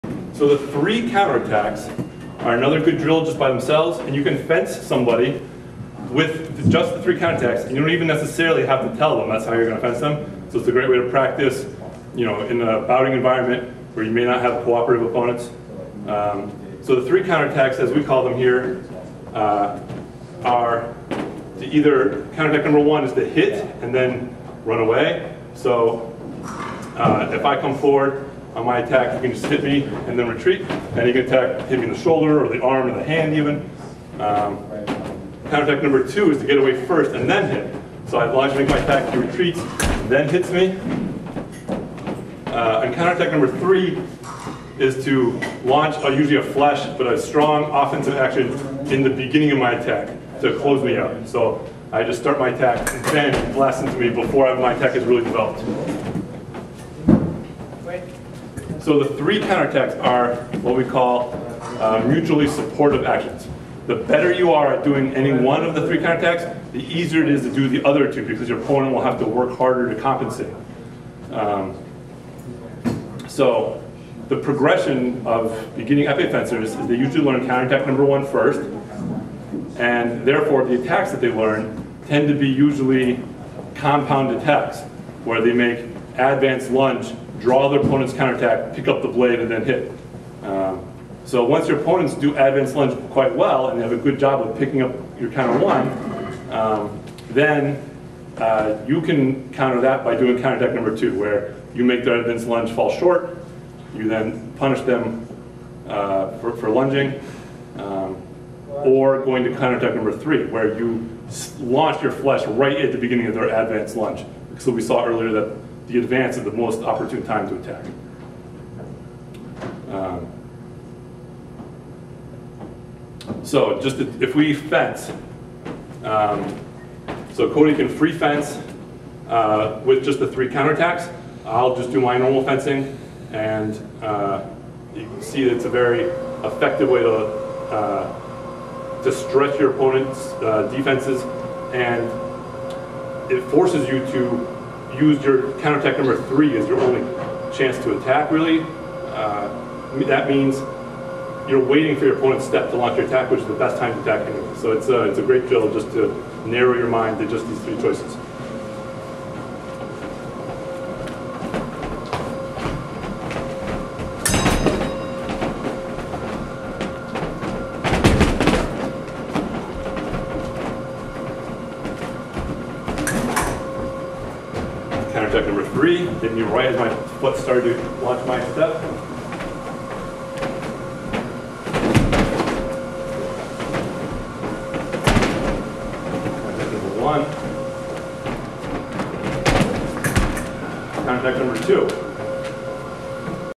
Epee Footwork Part 7 - the Three Counter-Attacks
Counter-attack #1 - Hit the arm and run away
Counter-attack #2 - Run away first (double retreat) then hit the shoulder
Counter-attack #3 - Hit and collapse distance by lunging or fleching to the shoulder on the opponent's preparatory action. Wait for them to commit to a compound attack or you may get counter-timed and they will parry-riposte.